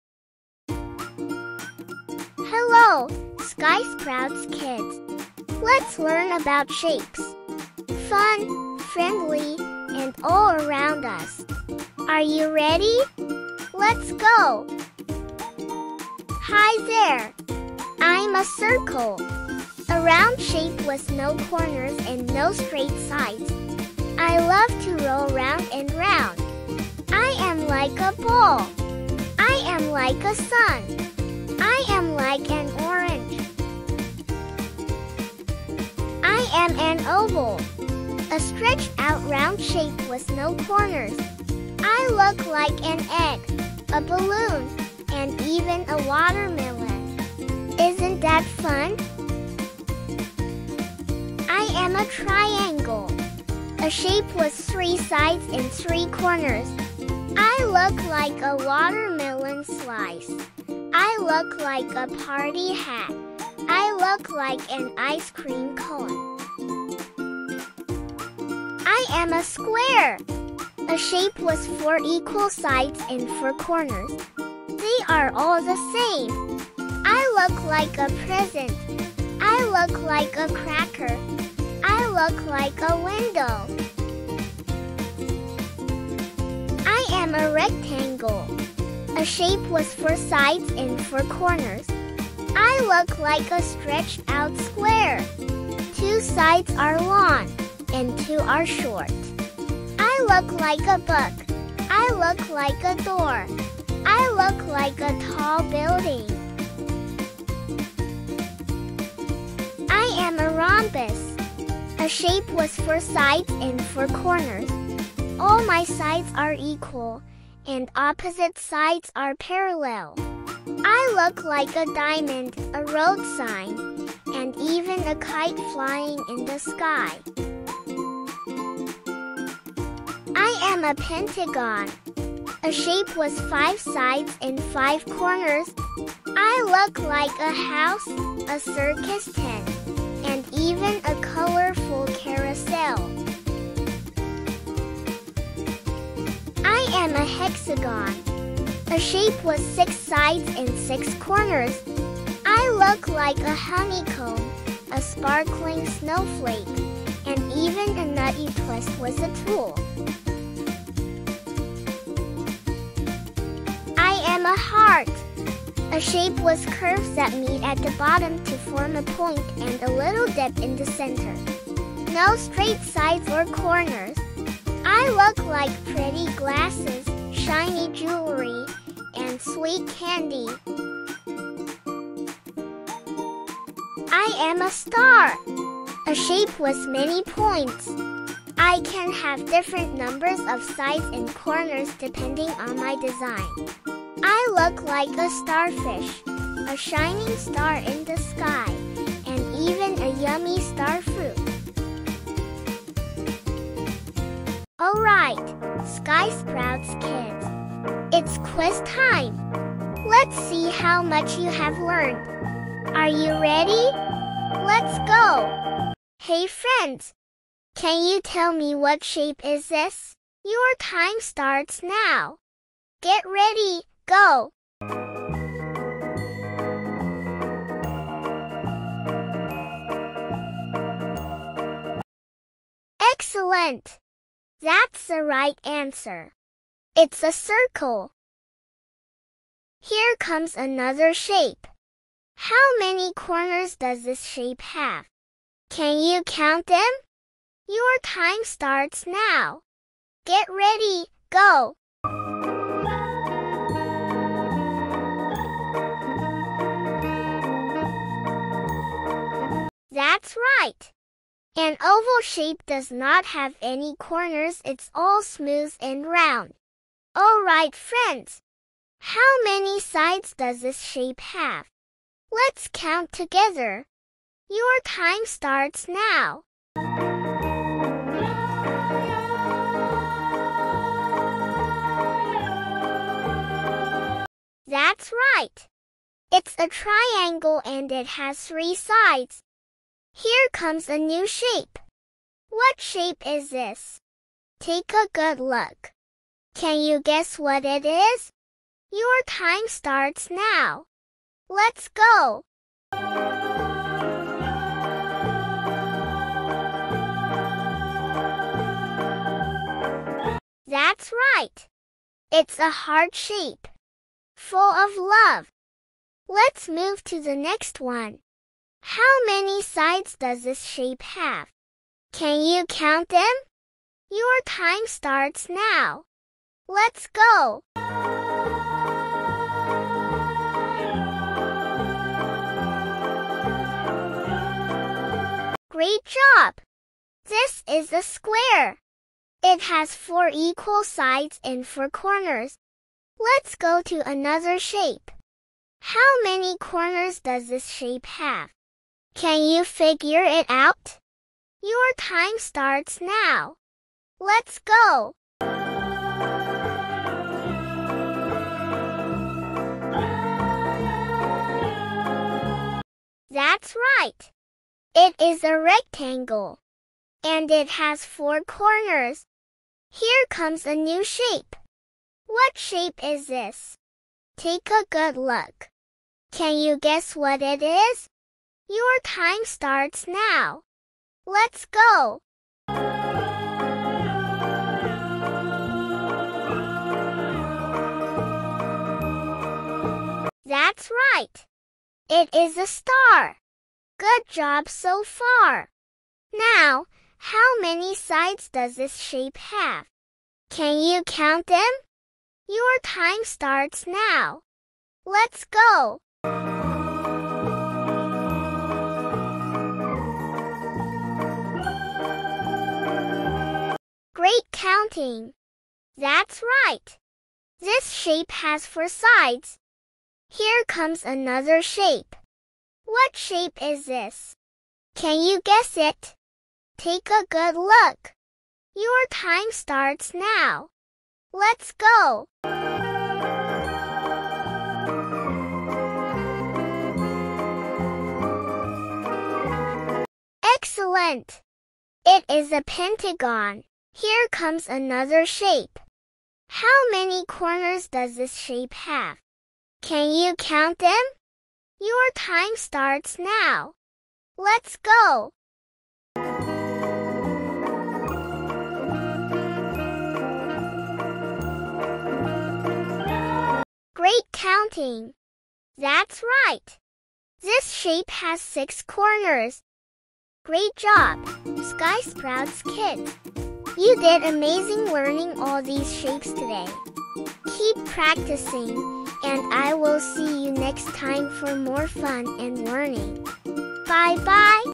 Let’s Learn Shapes + Fun Quiz Time! 🎉| Educational Video for Kids
🎉 Welcome to SkySproutsKidz!
Let’s learn all about shapes in a super fun and exciting way! 🟢🔺🟣 From circles to stars and hearts, we’ll explore different shapes — and guess what? There’s a quiz game too! 🎯

Can you guess the shape? How many sides? How many corners? 🤔
Join in, learn with us, and shout out the answers!

💡 Perfect for toddlers, preschoolers, and early learners.

✨ Shapes we’ll learn:
Circle, Square, Triangle, Rectangle, Star, Heart, Oval, Pentagon, Hexagon, and more!

📚 Skills covered:

Shape recognition

Counting sides and corners

Listening and thinking

Interactive learning

👶 Ideal for ages: 3–7 years
🎵 With fun voice prompts, music, and visuals!